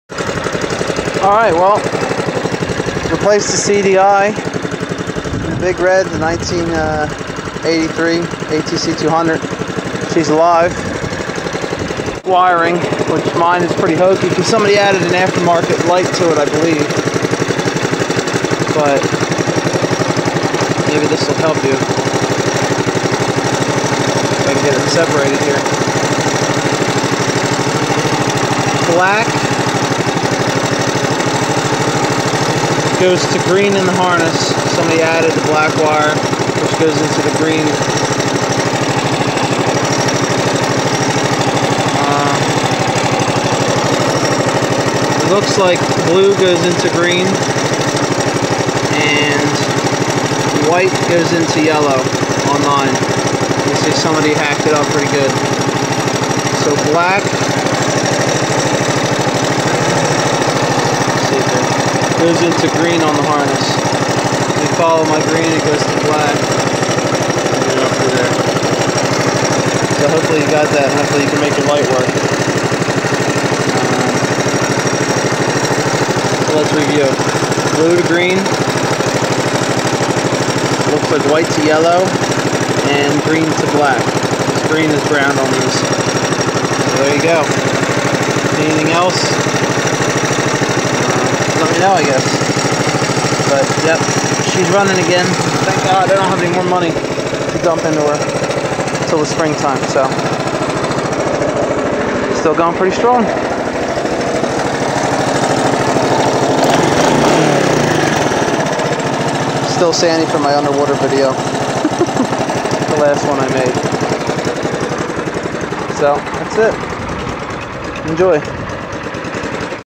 vmxrm85 headlight video.m4v
Video requested by vmxrm85 showing the headlight wiring of my honda atc200 big red.  Hope it helps buddy.